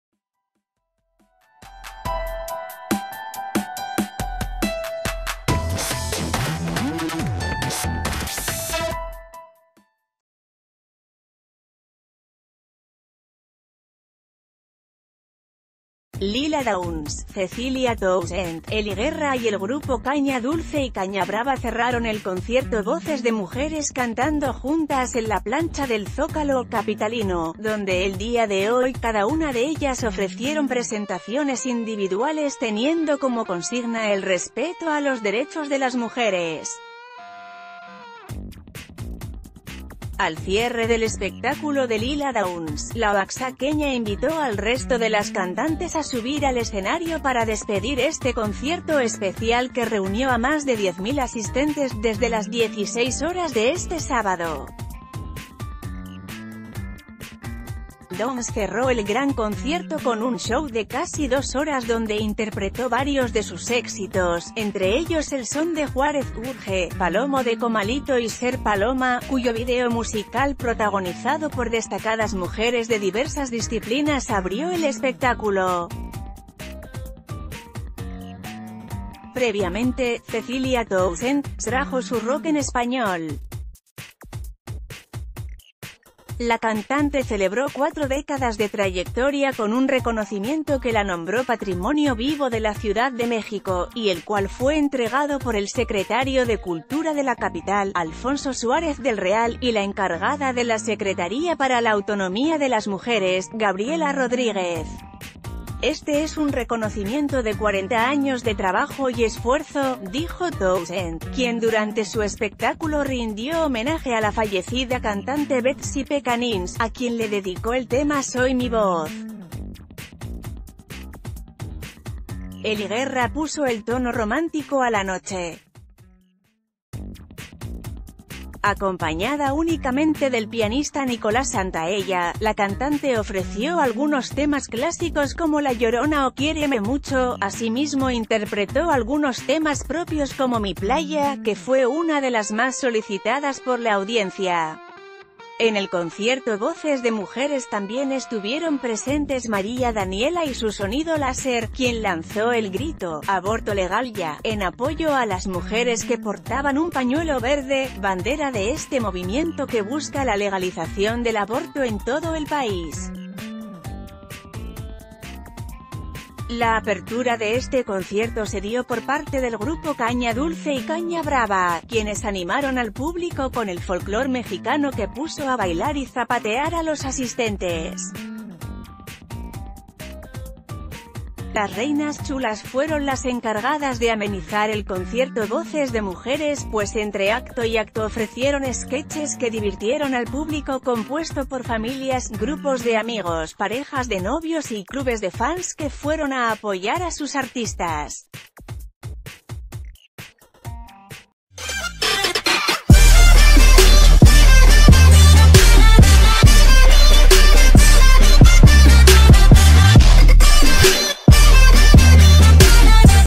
Con música, mujeres unen la voz por sus derechos
Lila Downs, Cecilia Toussaint, Ely Guerra y el grupo Caña Dulce y Caña Brava cerraron el concierto Voces de mujeres cantando juntas en la plancha del Zócalo Capitalino, donde el día de hoy cada una de ellas ofrecieron presentaciones individuales teniendo como consigna el respeto a los derechos de la...